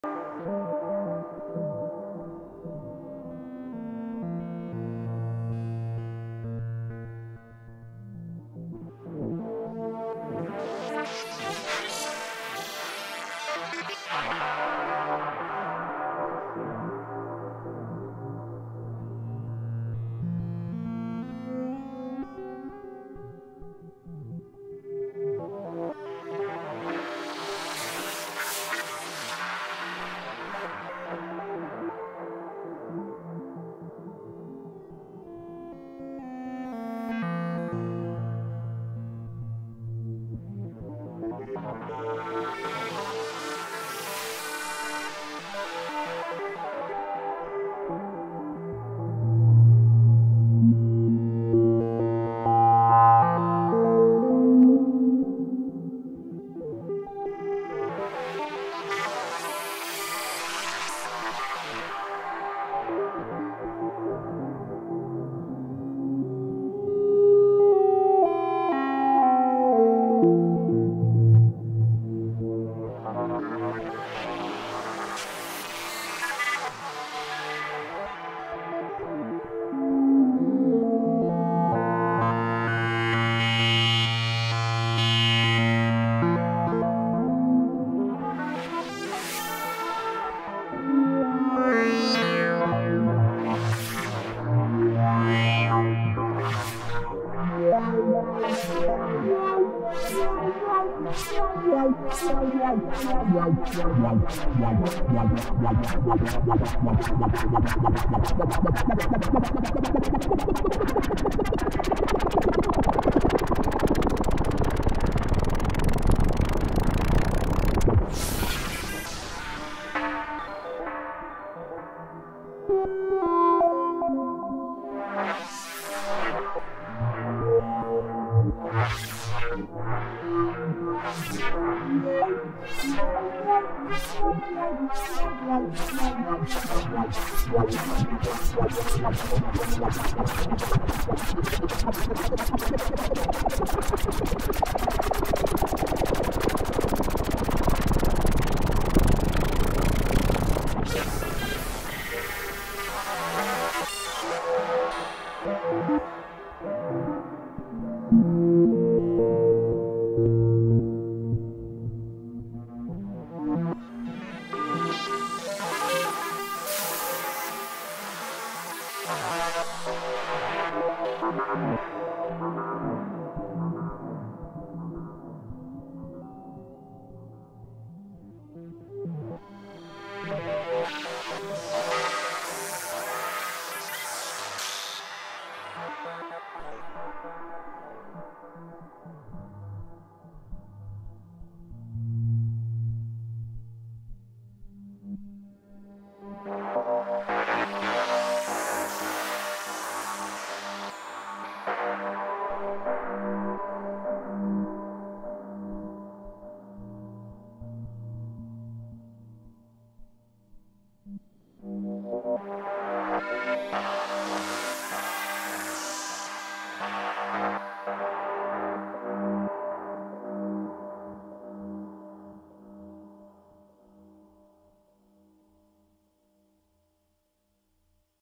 Piston Honda being all gorgeous!
Headphones on this one folks - STEREO manipulation!

USTA sequencing Piston Honda's two oscillators independently.  One oscillator running through Steve's MS-22 and the other through Zlob SVF.  Both filters modulated by Maths.

Modular experiments.